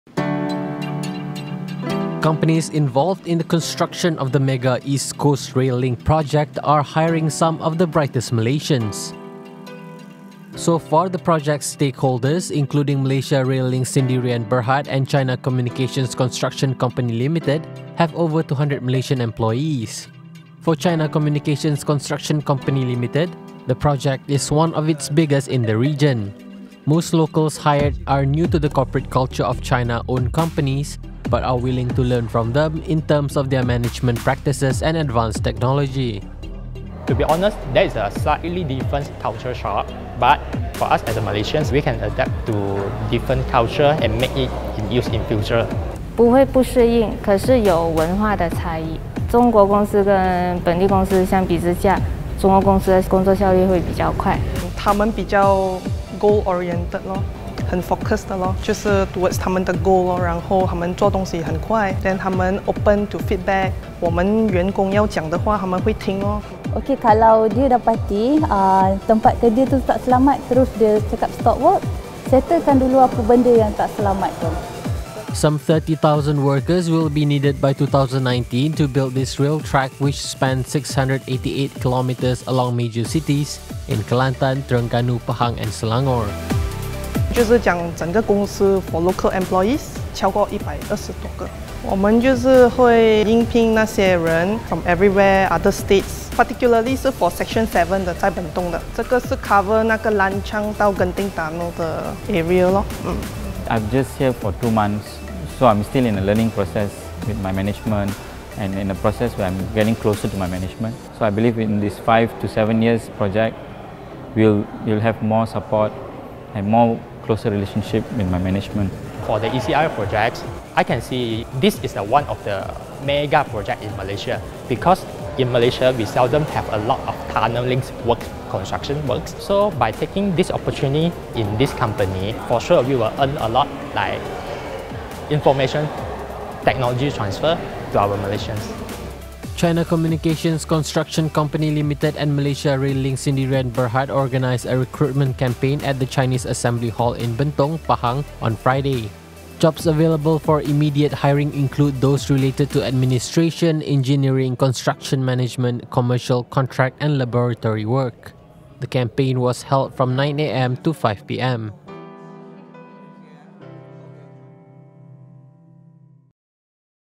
Malaysian employees at China rail company feel at home
China Communications Construction Co Ltd and Malaysia Rail Link Sdn Bhd, which are constructing the East Coast Rail Link project, have hired more than 200 local workers so far. It aims to recruit at least 70% local workers of some 30,000 workers needed for its peak period of construction in late 2019.